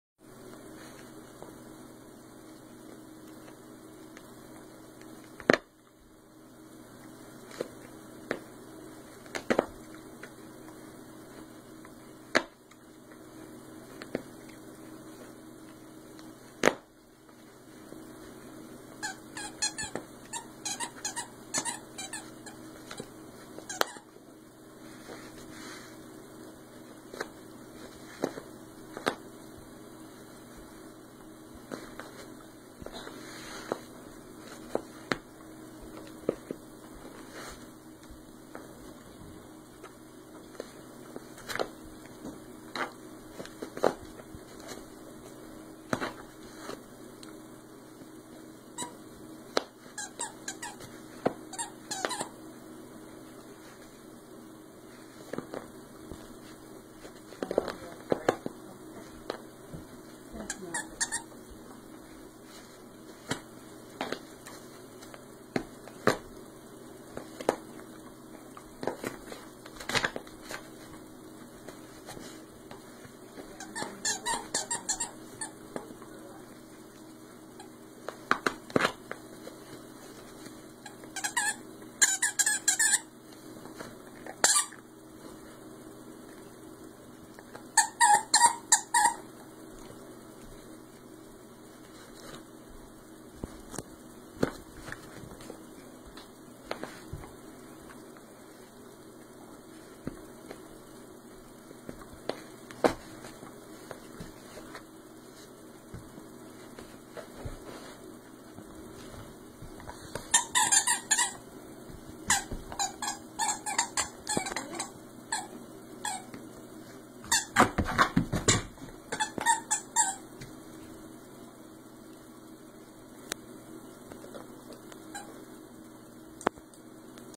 THE WOLF DESTROYS HIS PREY! My Husky Nova (Part 1)
My Husky Nova wants to go outside because there is snow out there. But we've already gone outside about ten times today so we're staying in and getting warm now. But he's still upset. So, he takes his frustration out on one of his toys (which he destroys in about 2 minutes flat). My mom recorded this video on her phone.